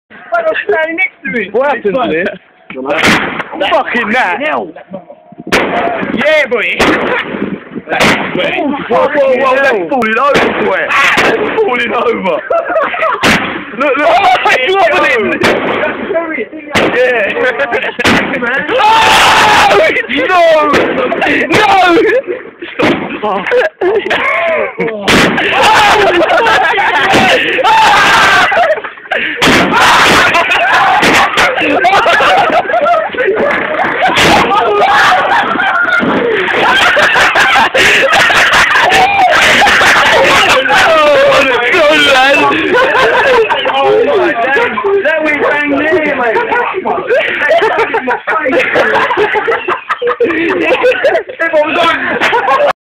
Firework goes wrong into crowd
Firework goes wrong into crowd after  not putting it in the ground properly